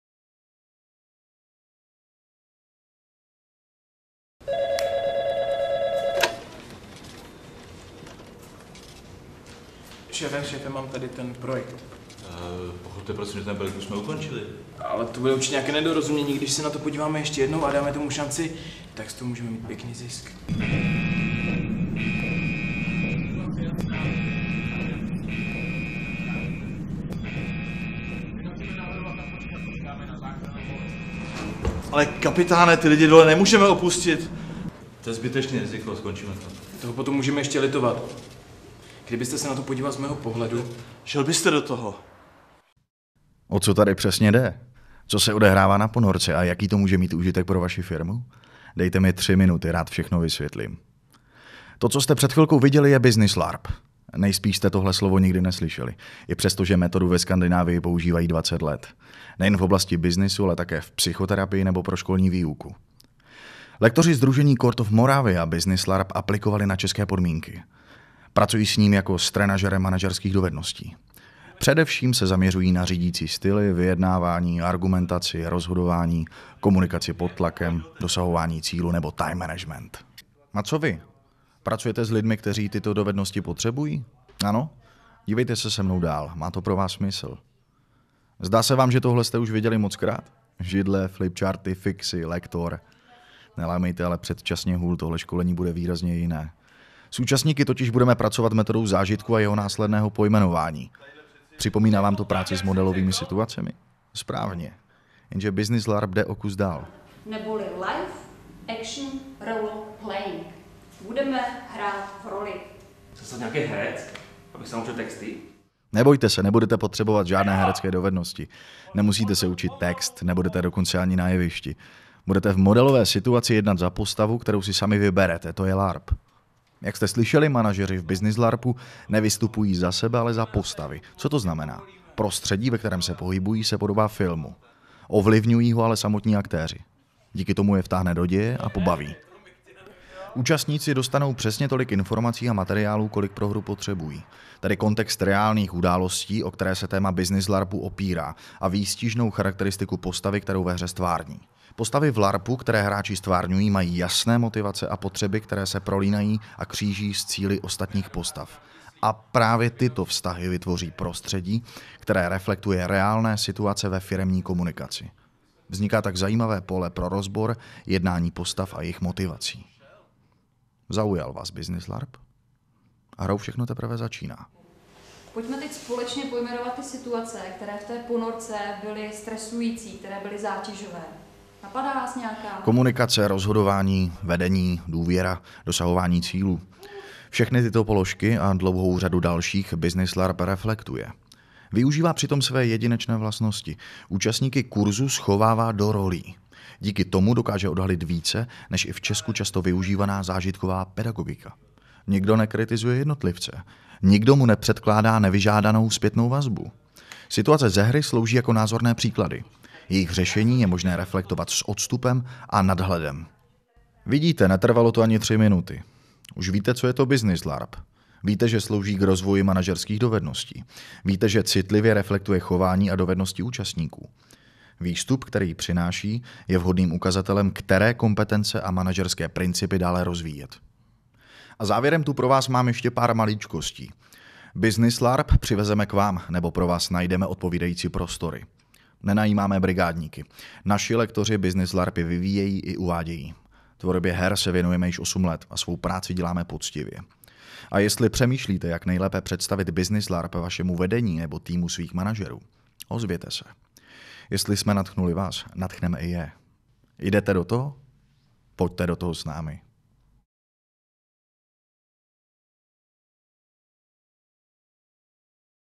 Co je to business larp?
Zajímáte se o business larp? Krátké video vám jej představí v pár minutách.

Máte zájem o business larp pro váš firemní tým?

Rádi zodpovíme vaše otázky, nabídneme možnosti a připravíme nabídku.